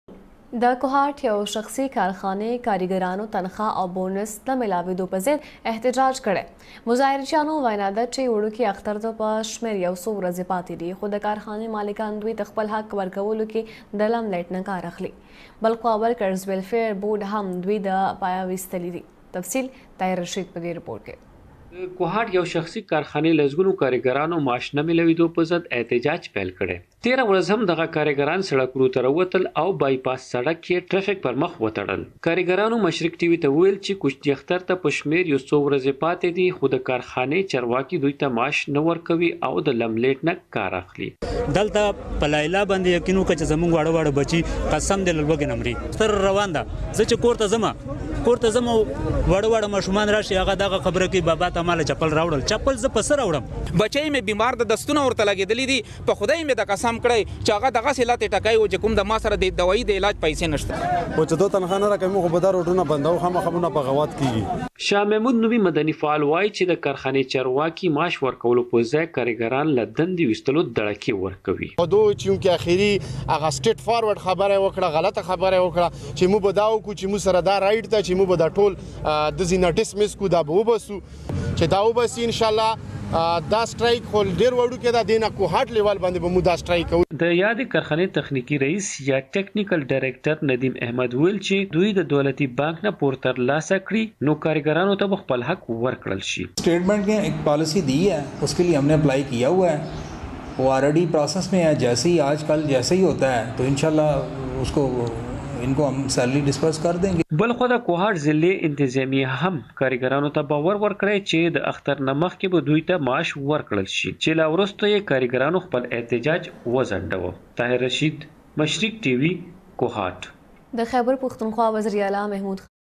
کوہاٹ : تنخواہوں کی عدم ادائیگی ،بابری کاٹن ملز لمیٹڈ کے درجنوں مزدور بونس سے بھی محروم،
کوہاٹ : تنخواہوں کی عدم ادائیگی ،بابری کاٹن ملز لمیٹڈ کے درجنوں مزدور بونس سے بھی محروم، ورکرز ویلفیئر بورڈ میں بھی شنوائی نہیں ہوئی مل مزدوروں کی مشرق ٹی وی سے گفتگو